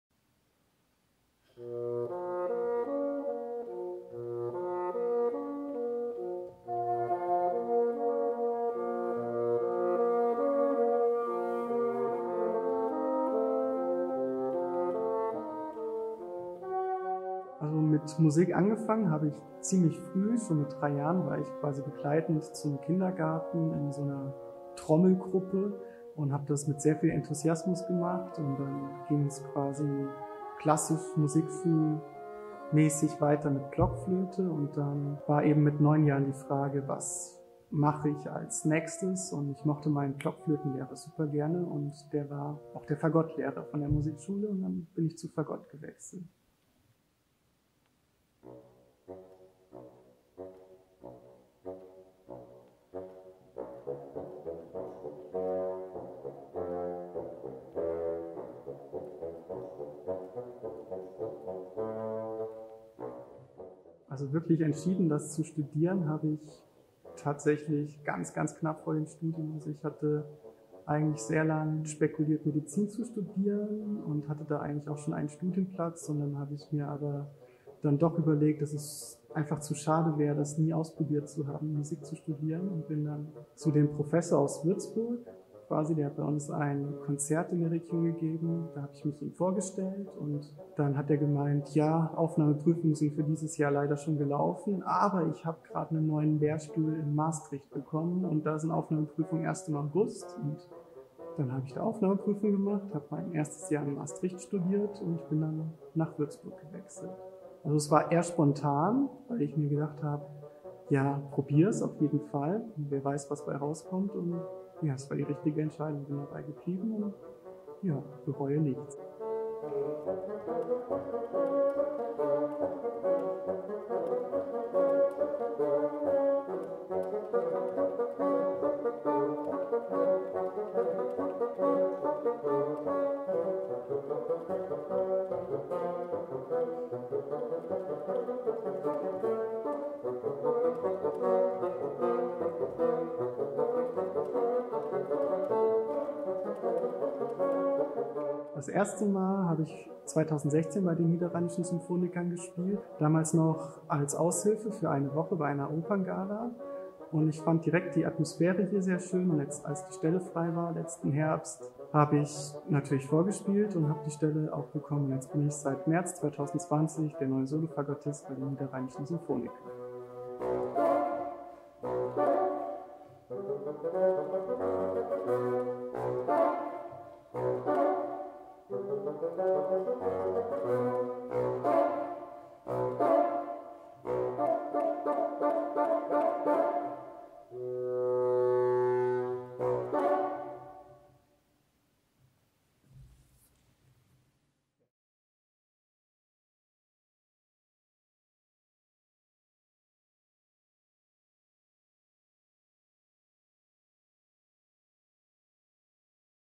Philipp Nadler - Solofagottist
Ein weiterer Kollege, der uns etwas über seinen persönlichen Hintergrund erzählt, ist unser Solofagottist Philipp Nadler. Wie er zur Musik, zu seinem Instrument und schließlich zu uns kam, erfahren Sie hier!